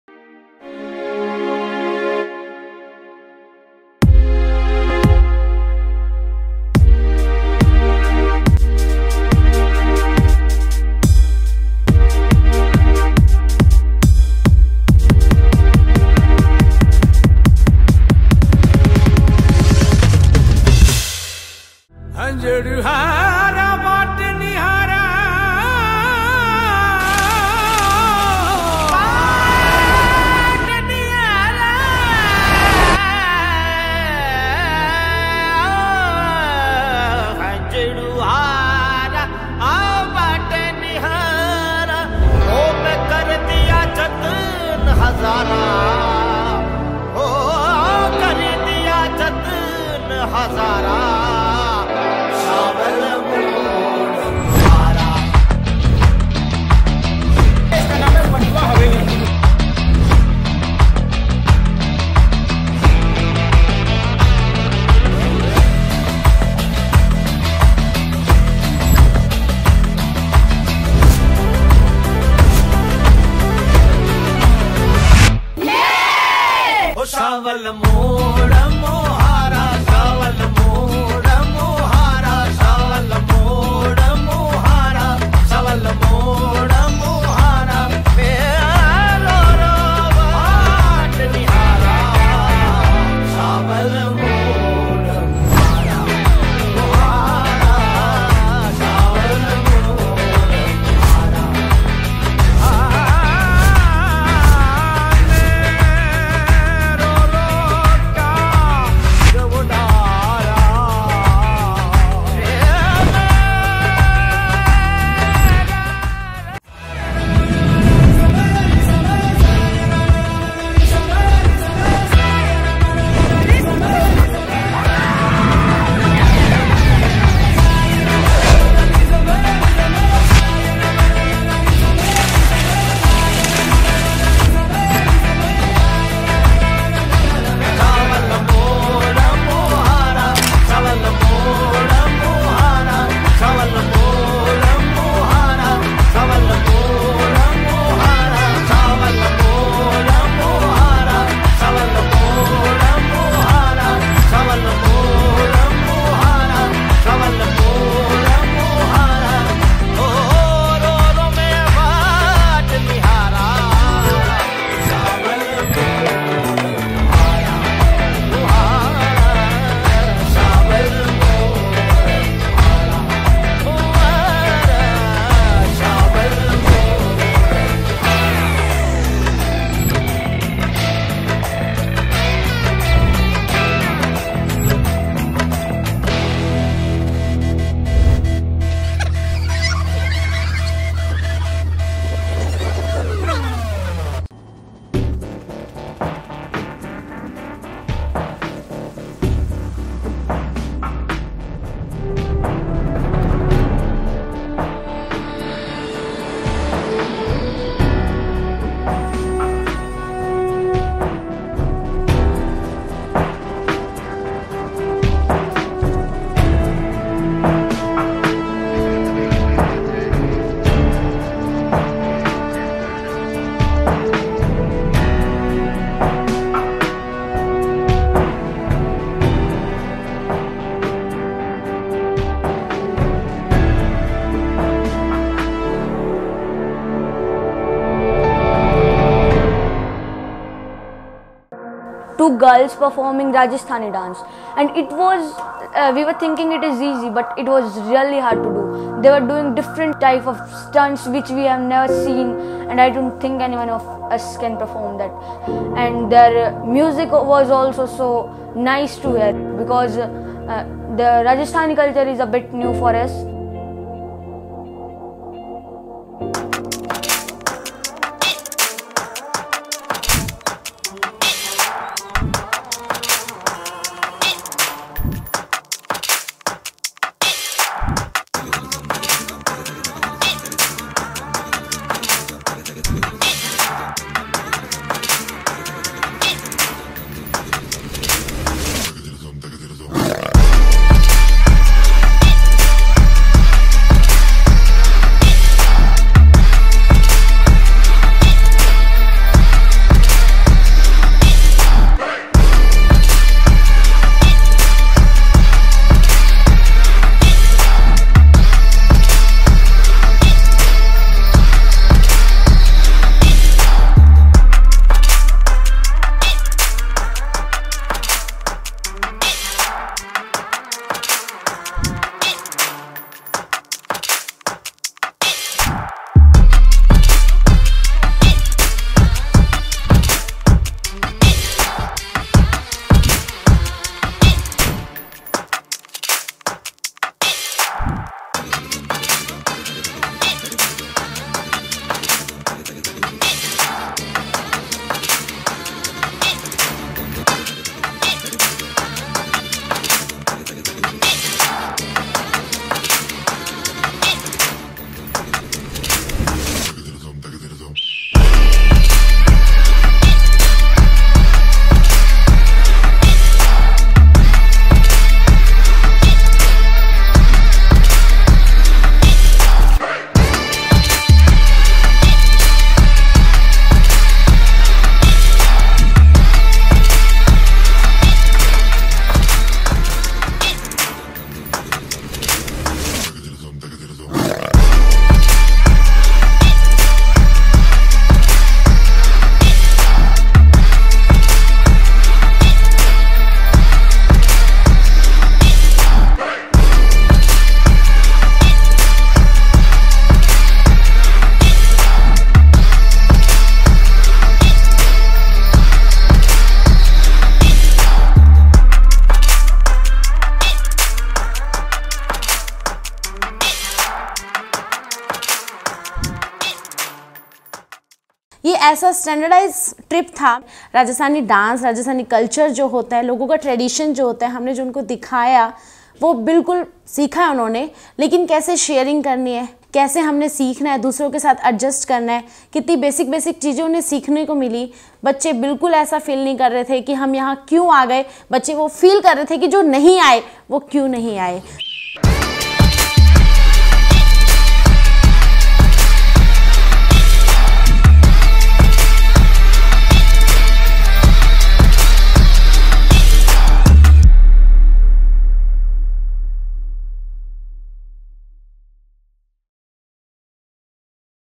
A Travel Documentary Film | Rajasthan Travel Video | Saraswati Vidya Vihar
Here is the much awaited video of our trip!
Our students visited Rajasthan in the first week of October, and brought home the feel of Rajasthani culture and amazing Rajputana heritage. Forts, palaces, sand dunes, desert camp and folk music and dance... almost everything in Rajasthan has a story to tell. The images of trip are vivid in our minds. In our attempt to relive our rendezvous with Rajasthan, colours of Rajasthan and thrill and joy of our trip captured by camera has been made into a travel documentary.